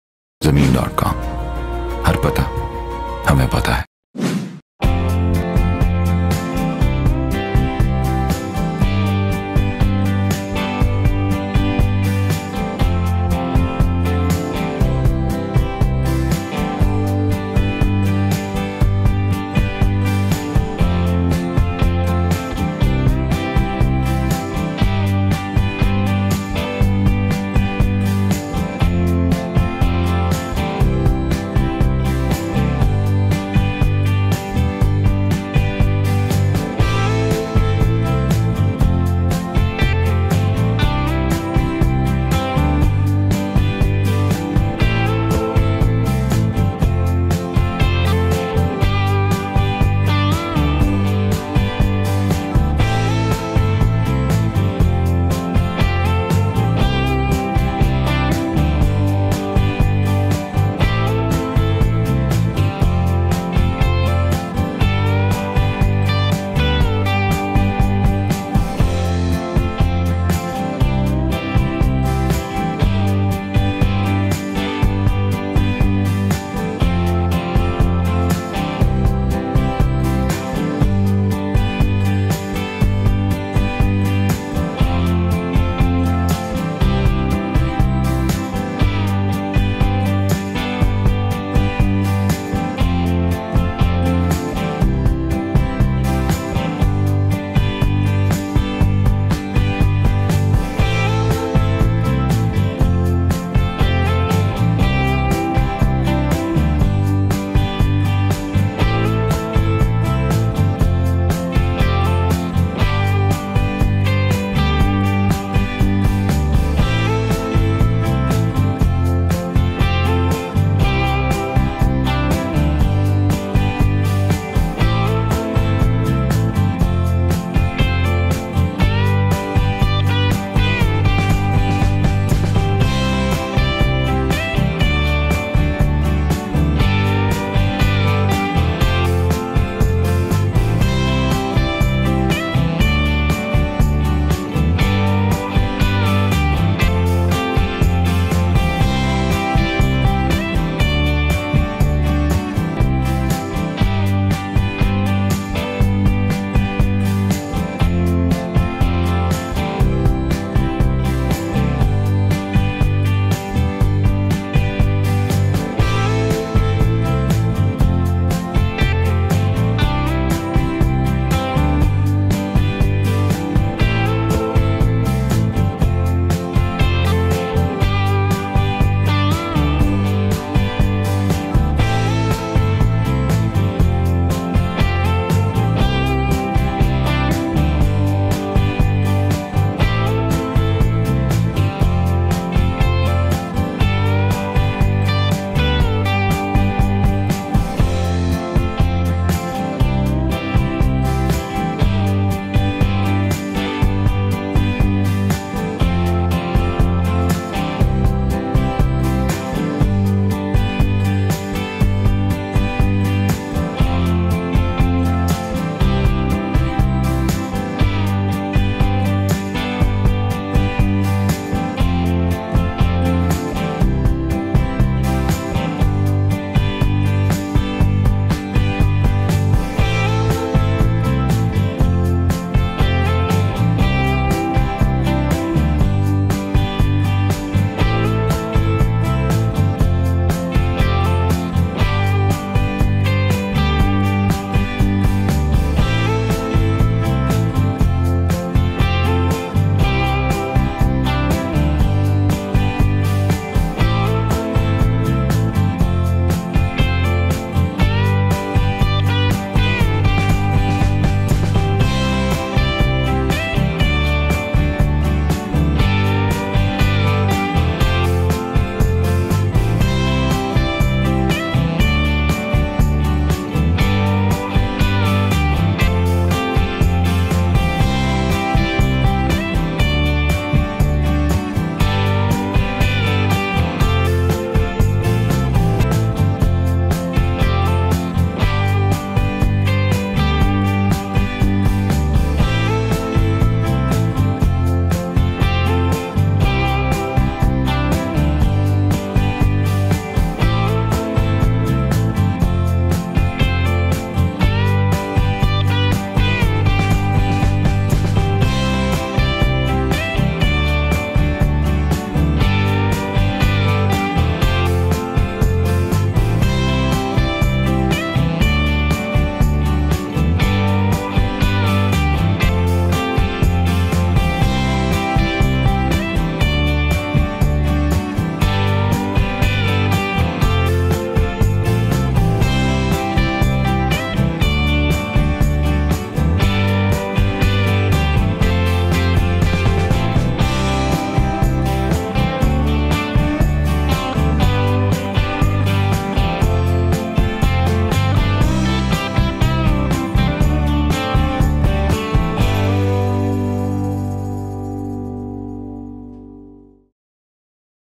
2 KANAL HOUSE FOR SALE IN PHASE 6 DHA KARACHI
Defence Phase-vi, 1000 Sq Yards Top Of The Line Brand New Bungalow, Designed By Well-known Architect Sharjeel & Anjum Parvez 7 Bedrooms, Spanish Tiled Bathrooms,through Out Italian Tile Flooring Rocca Grohe Fixtures /fittings,Teak Finish Pakistan Cables, Brande  Kitchen, Swimming Pool Basement, Theater, Gym, Lush Garden,Ample Parking,